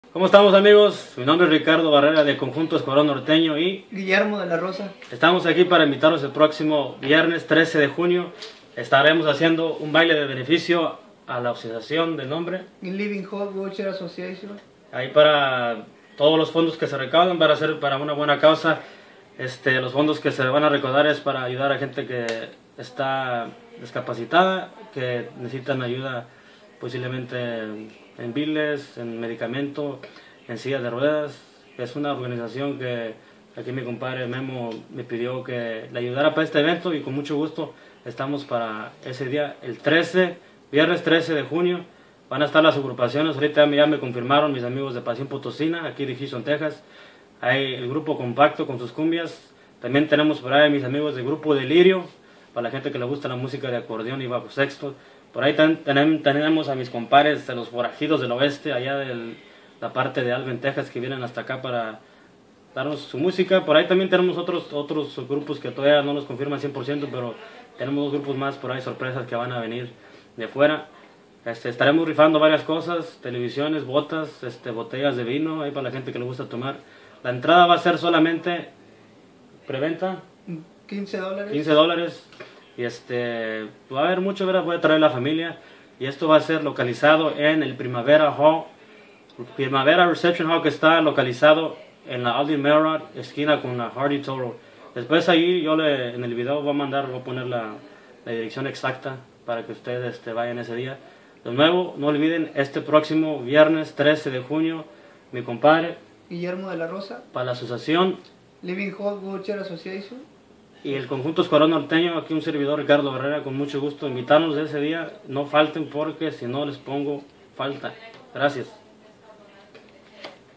Gran Baile-Viernes 13 de Junio-A Beneficio de Living Hope Wheelchair Association
Baile A Beneficio de Living Hope Wheel Chair Association.
Todos los fondos recaudados seran para ayudar a gente incapacitada, paralitica, con necesitades y medicamentos a sus impedimentos fisicos. Esperamos el apoyo de toda la gente de Houston, Texas

Primavera Reception Hall
1030 Aldine Mail Rd

Para mas informacion favor de llamar o text al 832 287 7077